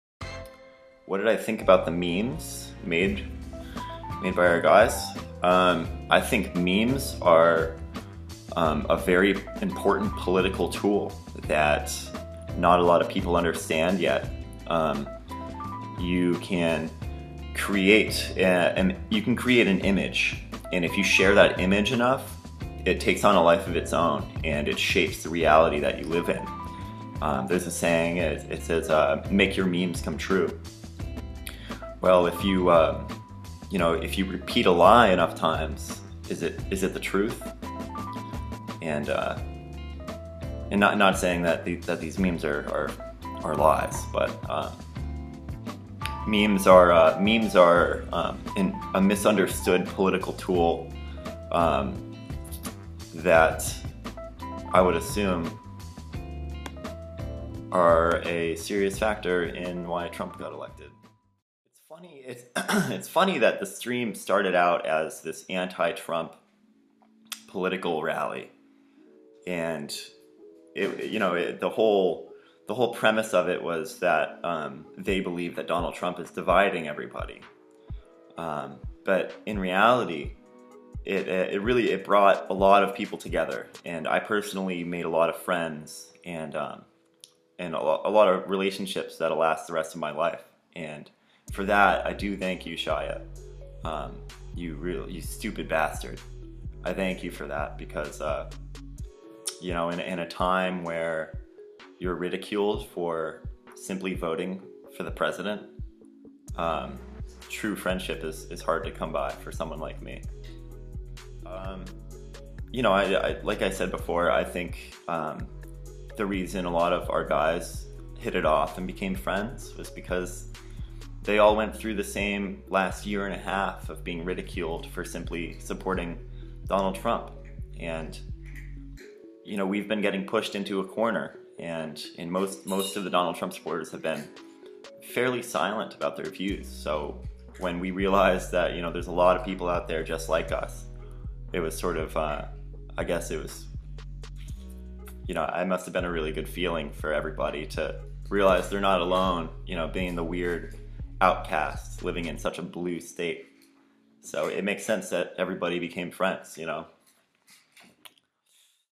Matt Colligan explains the massive psyop that was HWNDU and 4Chan
Most racists, bigots, etc., that identify as Republican have a common refrain: I have been abused for simply being a Republican and voicing my views.

What they don't mention is that the reason no one likes them is that their chosen leader is a bigot, and they are a bigot.

So that is another of their "deceit by omission" tropes. They love to leave out that they just happen to be horrible people. In any case, it's now 2023 as I write this and there's little question at this point in history about just how horrible the Republican party is. It's to a point that being a member of the Republican party, or voting Republican is an indefensible act.